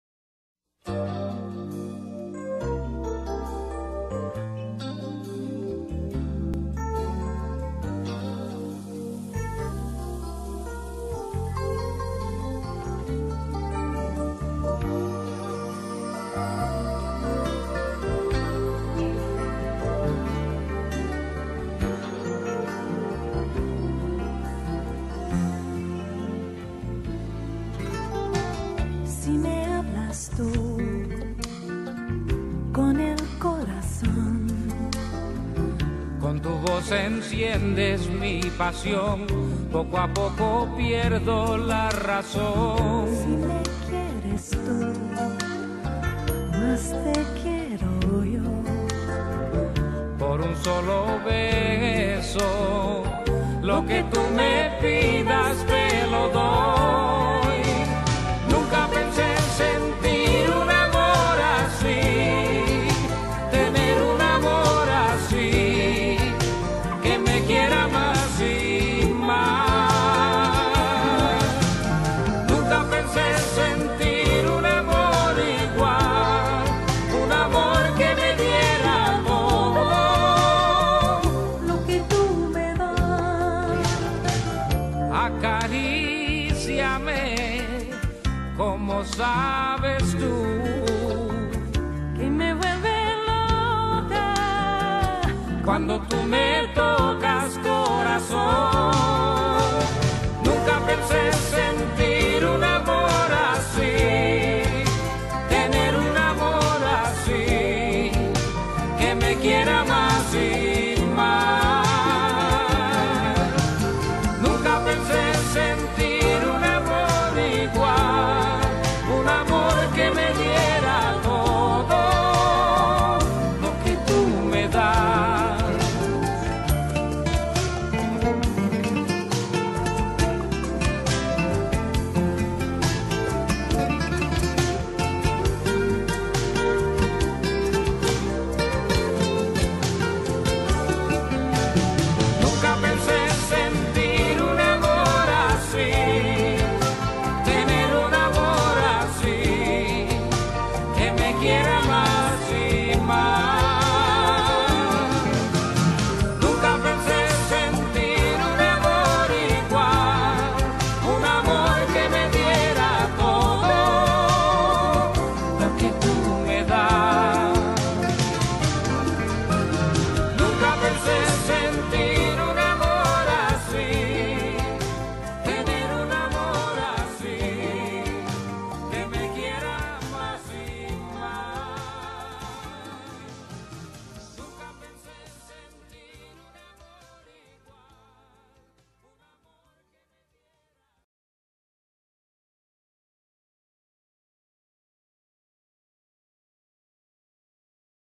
En New Jersey con mi esposa Ana Maria
Viaje de placer a New Jersey